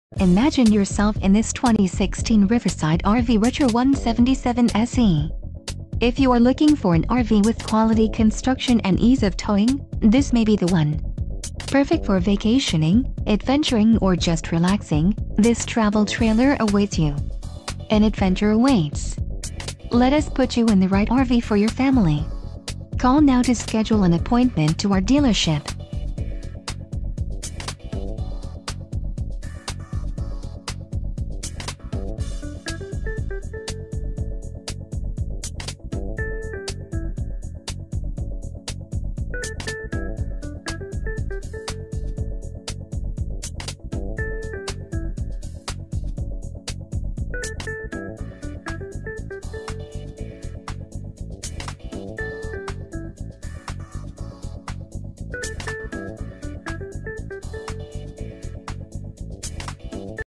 2016 Riverside RV Retro 177SE - Chilhowee RV Center - Lou...
This 2016 Retro travel trailer by Riverside RV is the model 177SE.SPECIAL LIMITED OFFER